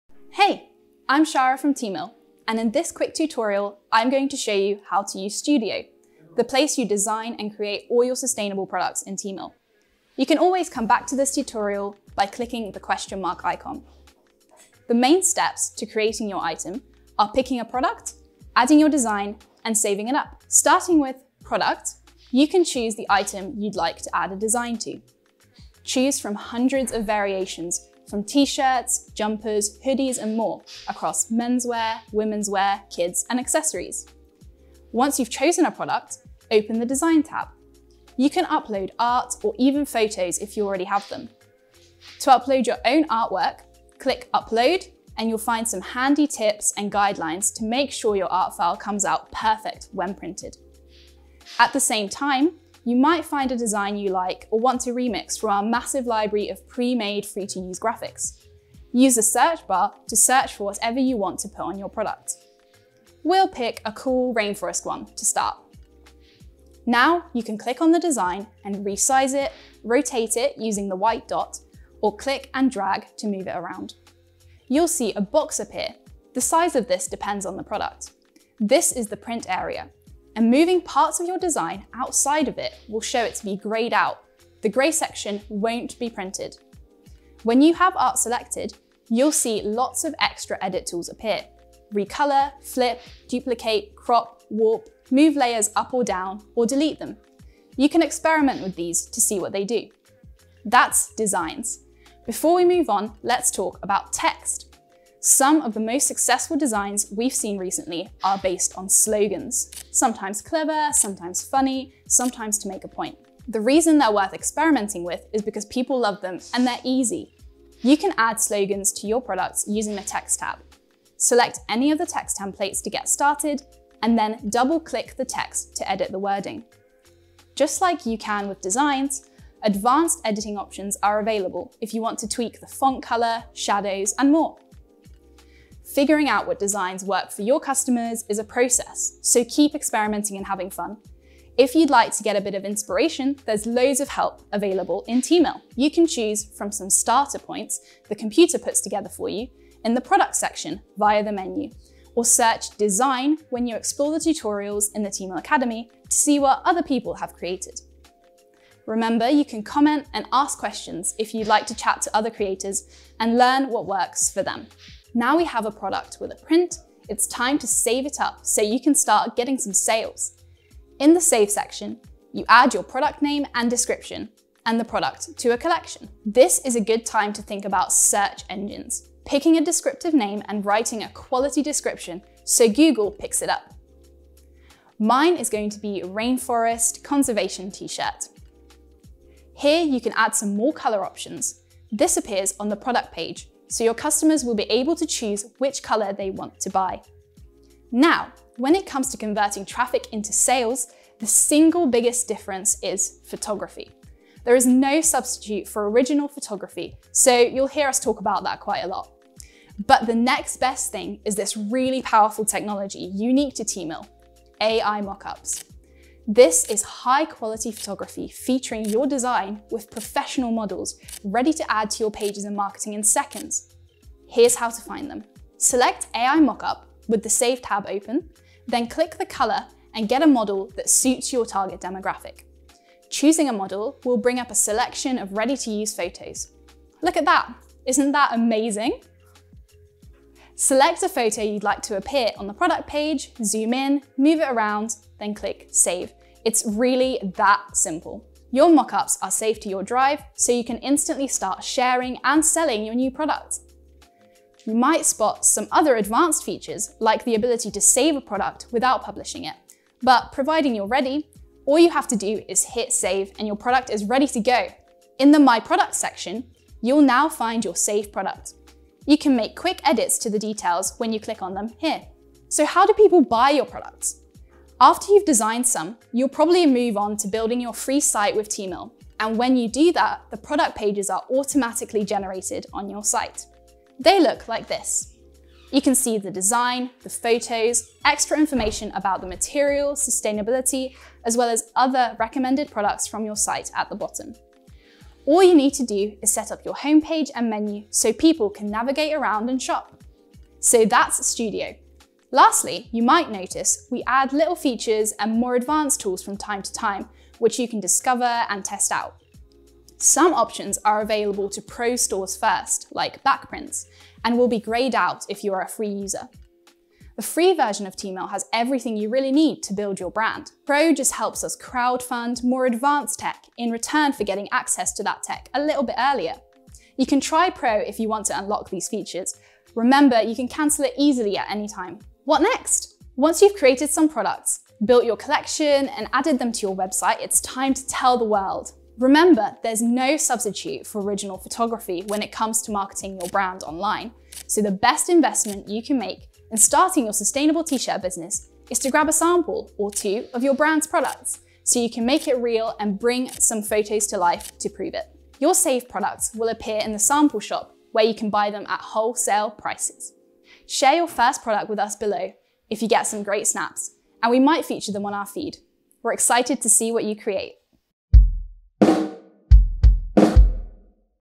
How to design sustainable products to sell online using Teemill Studio
In this quick tutorial, we show you how to design a custom t-shirt using Studio, the place you can create all your sustainable products in Teemill. All of the products you can design on have been made with natural materials like organic cotton. These products are stored as data and printed on demand when your customers need them so there's no waste. Plus, they're designed to come back and be remade into new products as part of our circular economy.

You can upload your own art or even photos if you already have them or choose a print you like or want to remix from our massive library of ready-made designs, free-to-use graphics, and slogan templates.

After you’ve designed some products, you can move on to building your free website with Teemill and when you do that, product pages are automatically generated on your site.

Create your own range of products and start your sustainable brand

Intro: 0:00-0:20
Choose your product: 0:21-0:33
Add artwork or a premade design: 0:34-1:41
Add slogans and text: 1:42-2:20
Design inspiration inside Teemill: 2:21-2:44
Optimise your product for search engines: 2:45-3:16
Add AI mockup photography: 3:17-4:13
Add your products to your free website: 4:14-4:59
Advanced features inside Teemill Pro: 5:00-5:31
Snap some photos with samples: 5:32-6:11